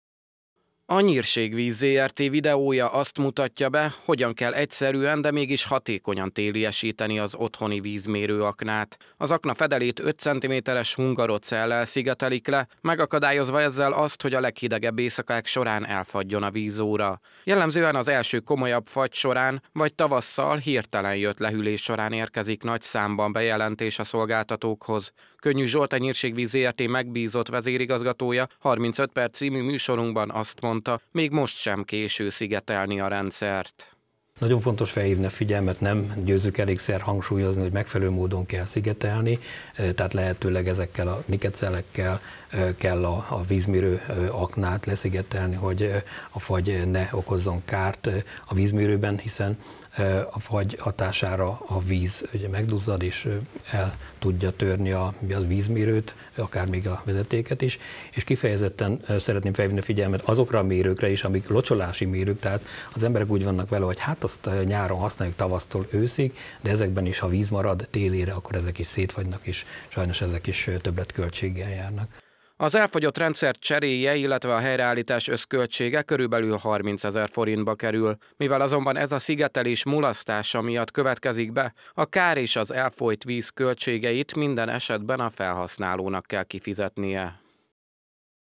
Még nem késő téliesíteni
Még most sem késő téliesíteni, azaz megfelelően szigetelni a vízmérőaknákat – erre hívja fel a figyelmet a Nyírségvíz Zrt. Telente rendszerint több száz bejelentés érkezik a szolgáltatóhoz elfagyott vízóra miatt. Érdemes ugyanakkor néhány percet rászánni az akna szigetelésére, a kár helyreállítása ugyanis több tízezer forint, amelyet a felhasználónak kell kifizetni.